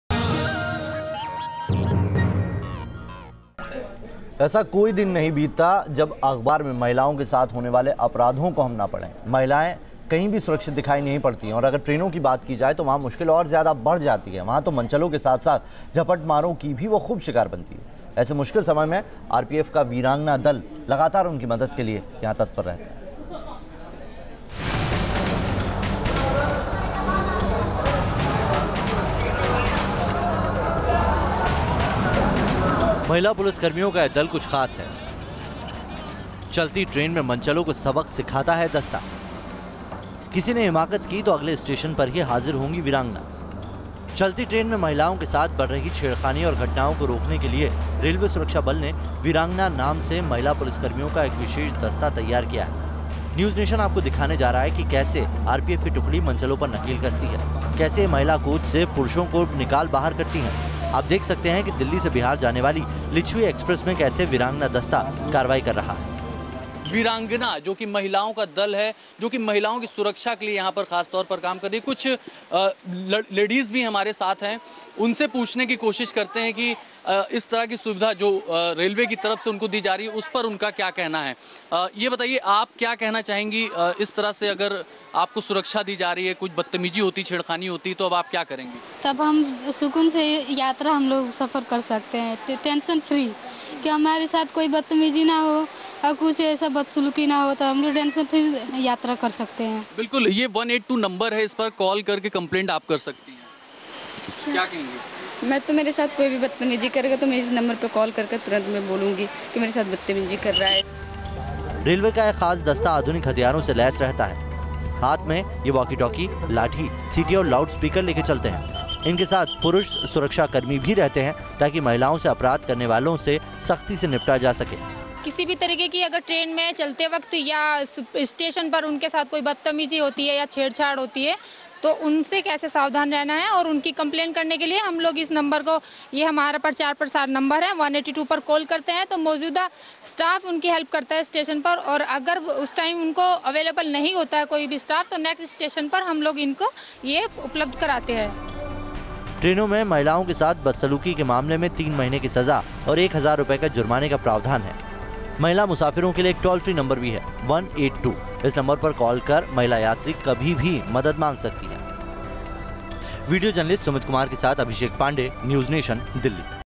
special report: Women squad of RPF called 'Veerangana' helps the women in train
A special women wing of Railway Police Force (RPF) called 'Veerangana' is helping the women in the train when any wrong situation ouccured with them. Watch this special report of women RPF team of railway.

News State is the most watched of Uttar Pradesh and Uttarkhand, available on various broadcast platforms like Tata Sky, Den, Hathway etc.